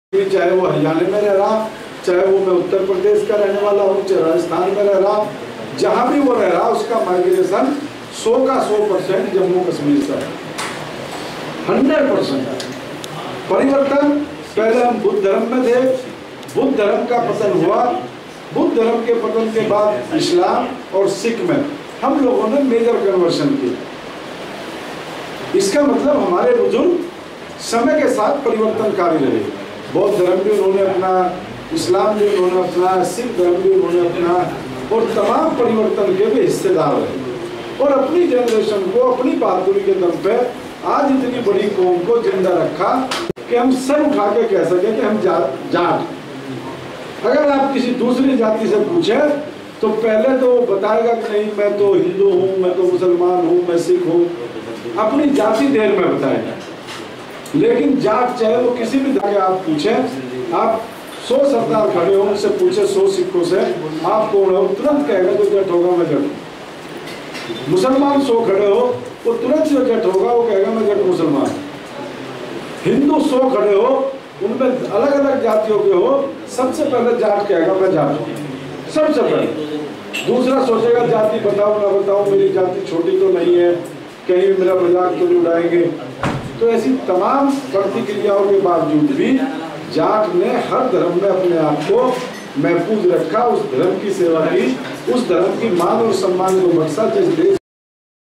1 june 2011 जाट संकल्प यात्रा जम्मू एंड कश्मीर
१ जून २०११ को जाट संकल्प यात्रा कठुआ जम्मू पहुंची